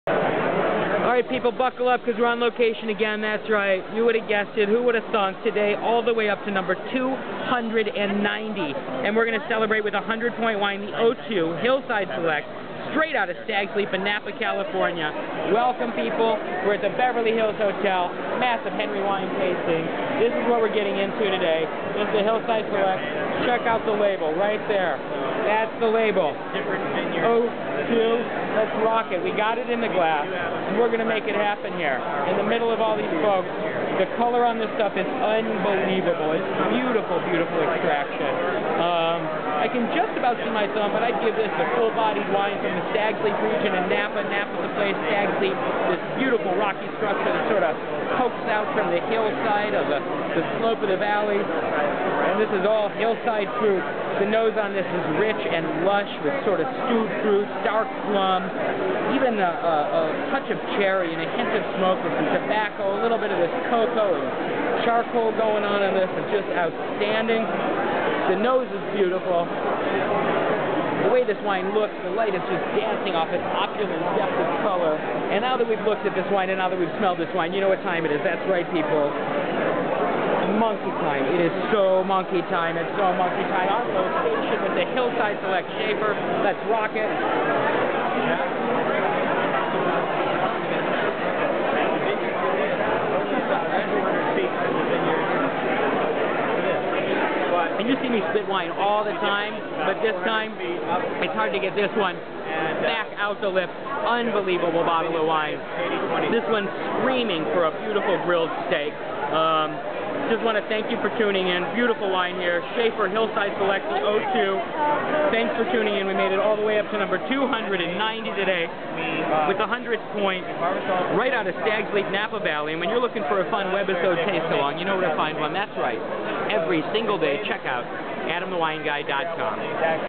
Taste Along w/ Adam The Wine Guy #290 - Shafer Hillside Select
ON Location tasting the 2002 Shafer Hillside Select , Cabernet Sauvignon, from Napa.